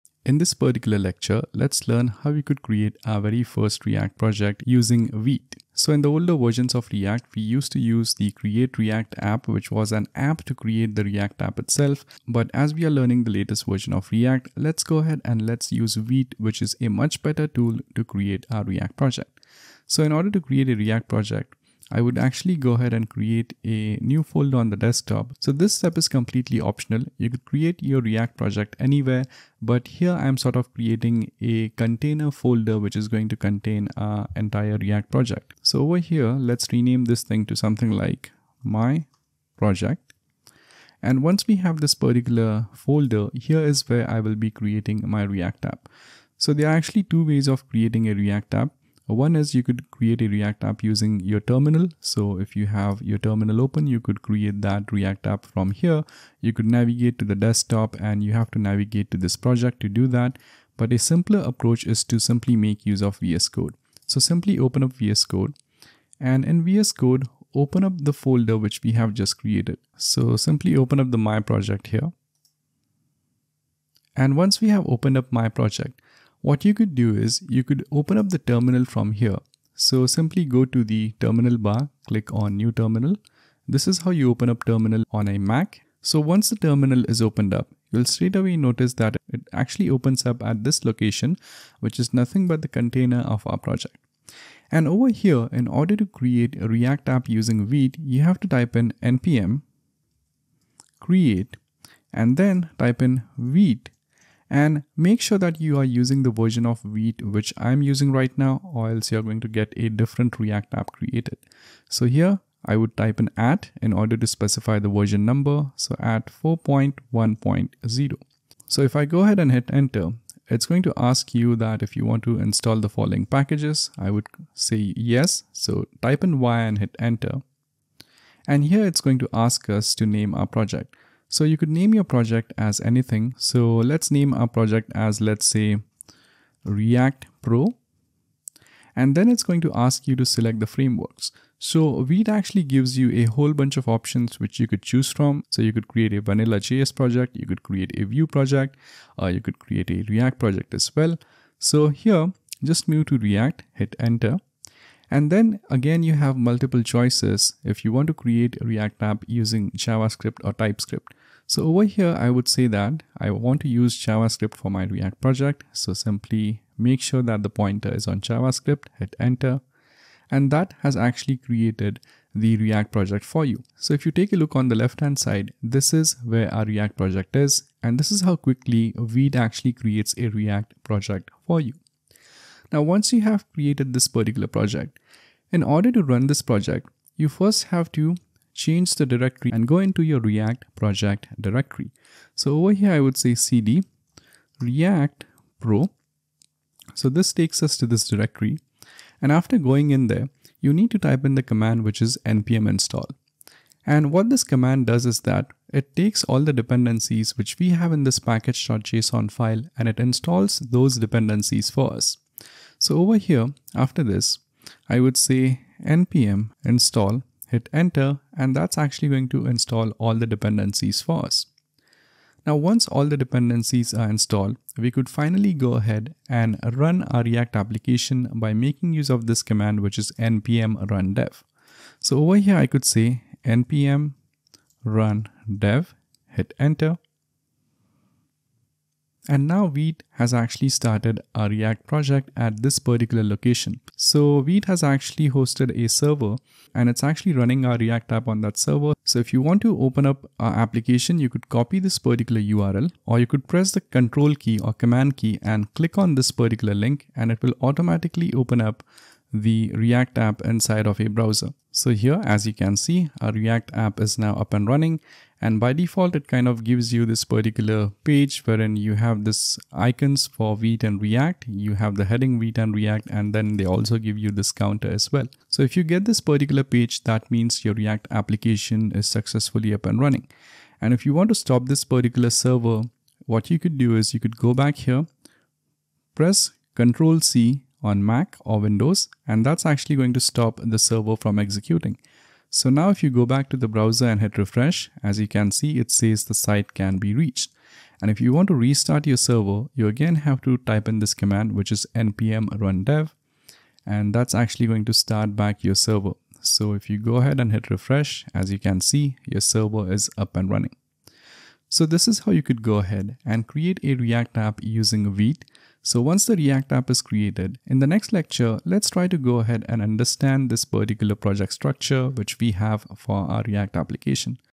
ReactJS Tutorial 3 : Create React App Using VITE
This is how to create a React app using Vite.

Firstly, create a react project using Vite.

Create a folder on desktop and open it up in vscode.

Open terminal in vscode and type in

``npm create vite@4.1.0```

Run the project from VS code instead of the terminal.

Back in the day we used to create React applications using the create react app (CRA) tool which is no longer the suggested way to create React apps, instead we now use Vite which is a much faster tool to create React applications.